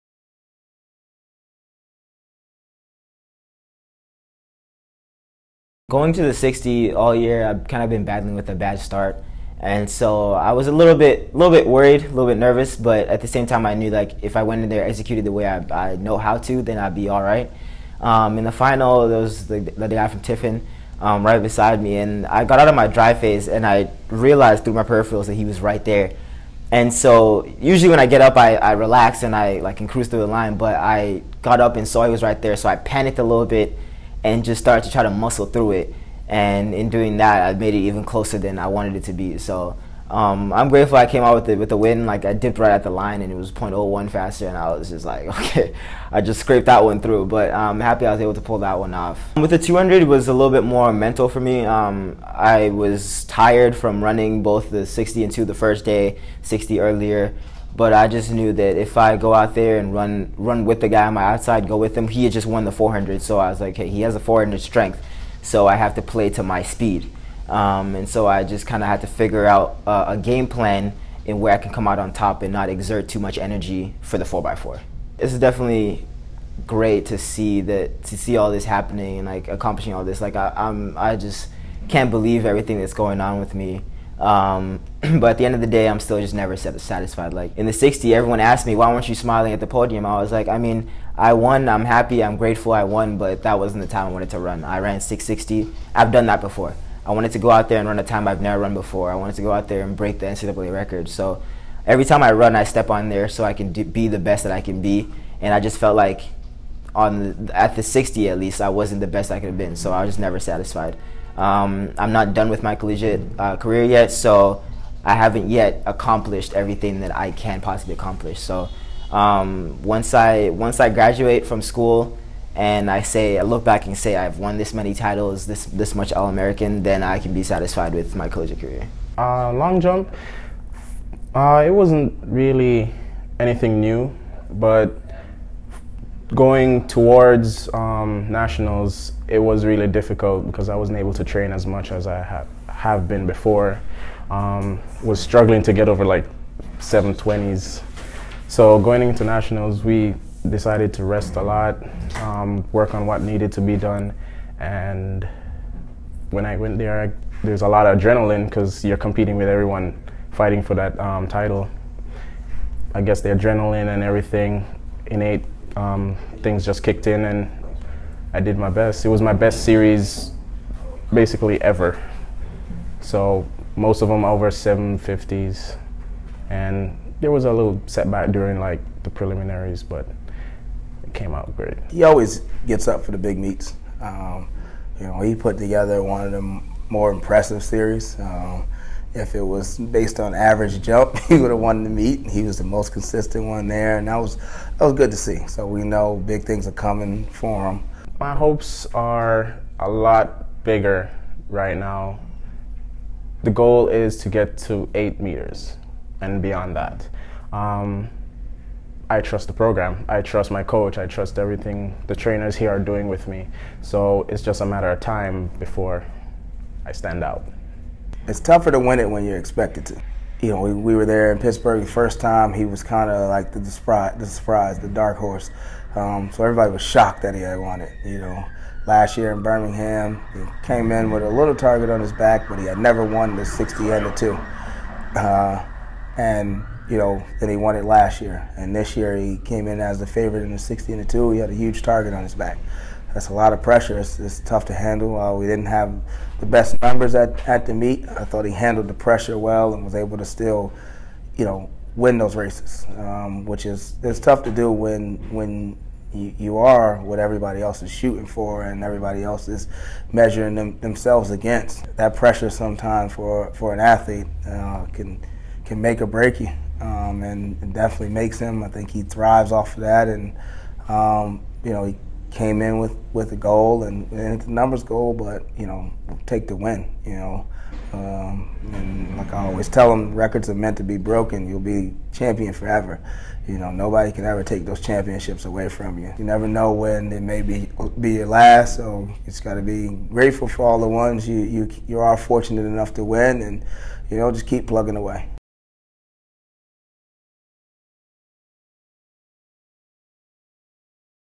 Track & Field Weekly Report 3-14-18
Academy of Art University​ redshirt junior sprinter Mobolade Ajomale talks about defending his NCAA Division II​ titles in the indoor 60m and 200m, senior jumper Michane Ricketts recaps back-to-back DII All-American honors in the indoor long jump, and head coach Kevin LaSure discusses their performances at NCAA DII Indoor Nationals in Pittsburg, Kansas on Mar. 9-10.

Video edited by Academy of Art Communications & Media Technologies major/Baseball alum Brett Bovee.